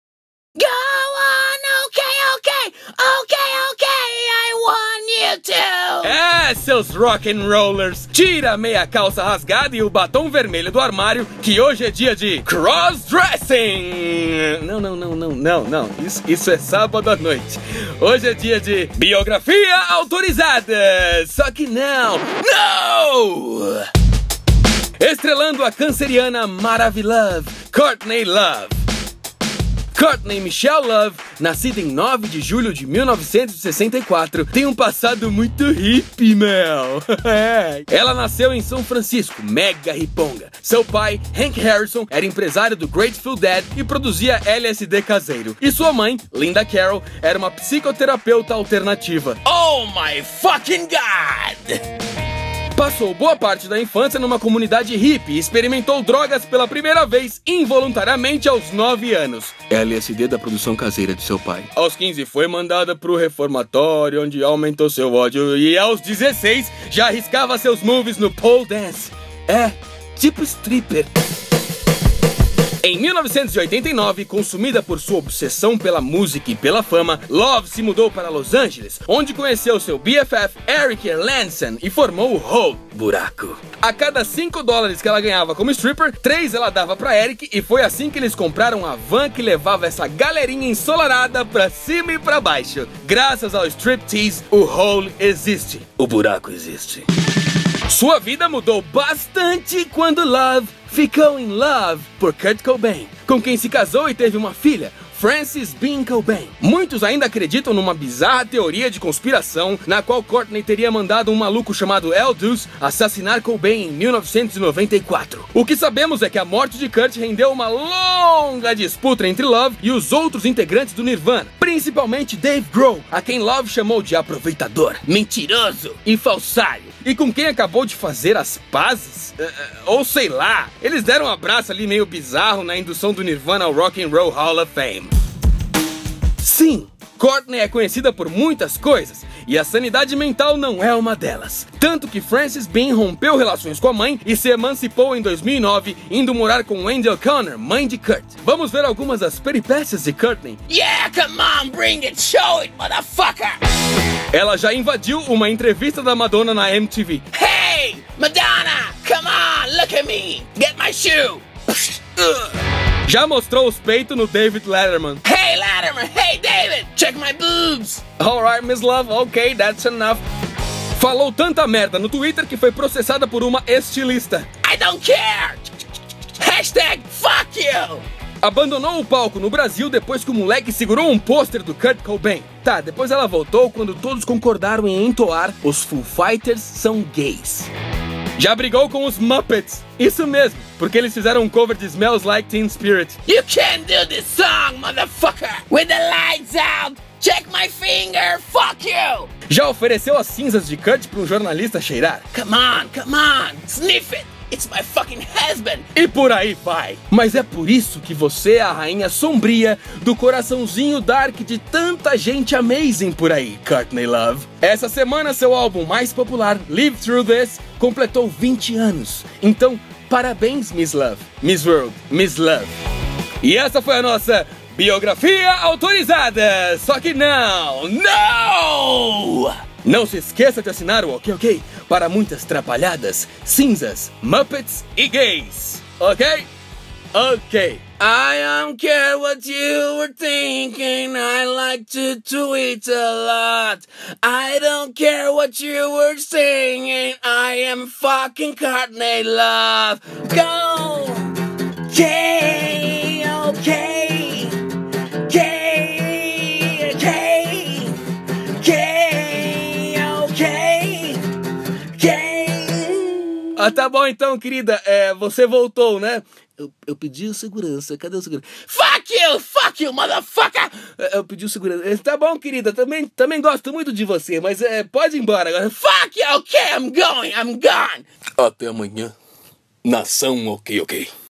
FOFOCAS DA SEMANA
Bom dia, ia ia ê ê ê!

Hoje é dia de Biografia Autorizada (Só que Não) e nesse caso não mesmo, pois vamos falar de Courtney Love. Estamos até com um pouquinho de medo de encontrar ela na rua, sei lá, né...

Go on, watch everything, watch everything!

A vida autorizada dos famoso. Só que não. Nem

4+1 FOFOCAS DA SEMANA - SEXTAS @ 11H